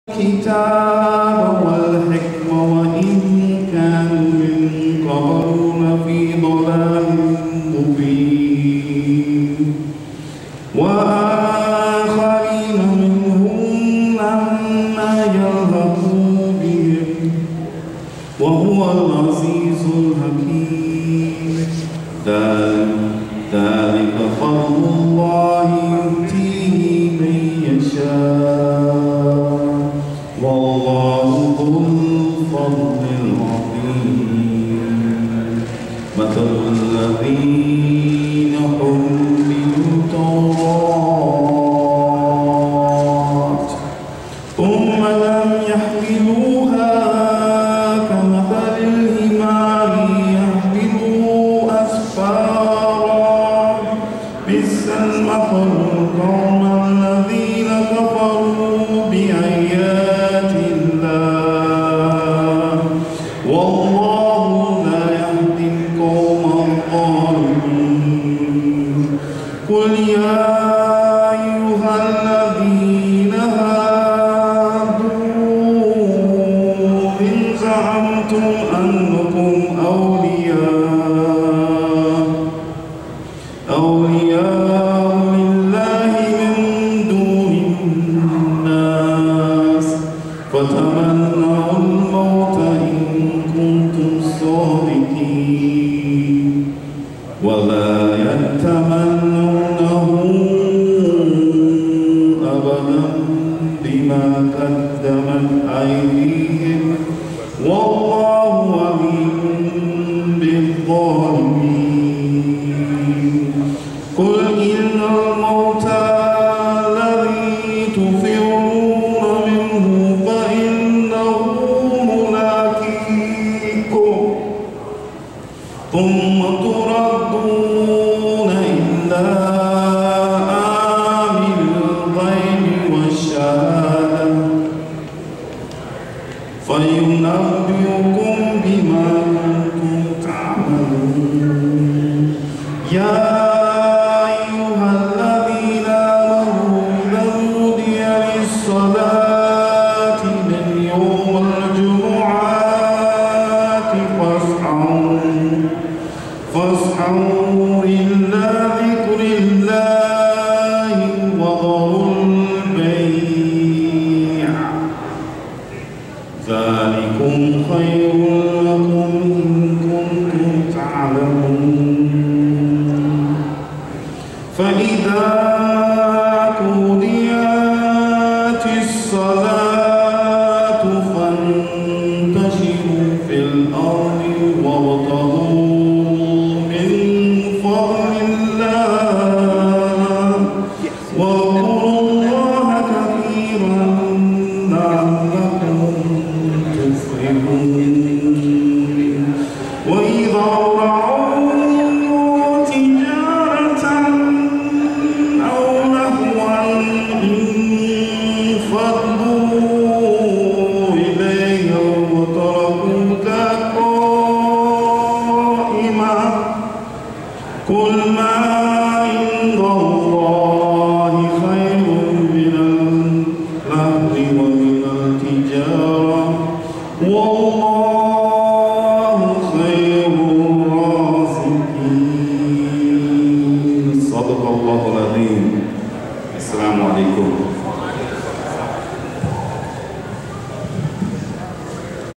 Quranic Arabic Recitation - Imam Askia Muhammad
The Mosque Cares 27th Annual Muslim Convention